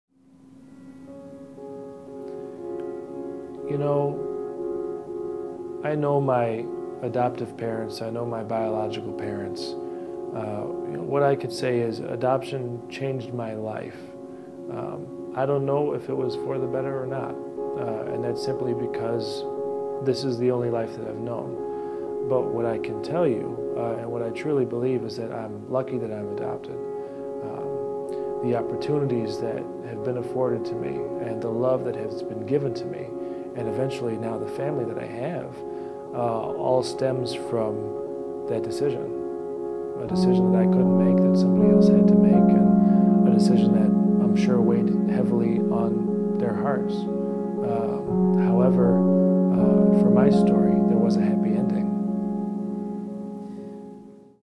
Olympian Aaron Parchem on Being Adopted
In honor of National Adoption Month, American Adoptions met with Aaron Parchem, pairs figure skater in the 2006 Olympics in Torino, Italy. Aaron was adopted as a baby and points to his adoption and his adoptive parents as reasons he was able to proudly represent his country in Torino.